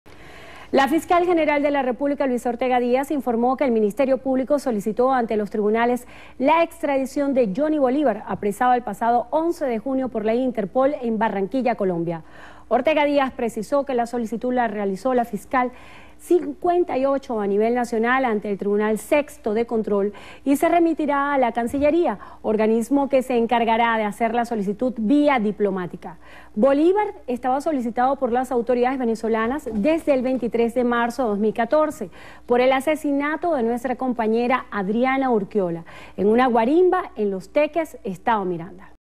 Fiscal Ortega Díaz indicó que fue solicitada la extradición de Jhonny Bolívar
La fiscal general de la República, Luisa Ortega Díaz, informó que el Ministerio Público solicitó ante los tribunales la extradición de Jhonny Bolívar, apresado el pasado 11 de junio por la Interpol en Barranquilla, Colombia.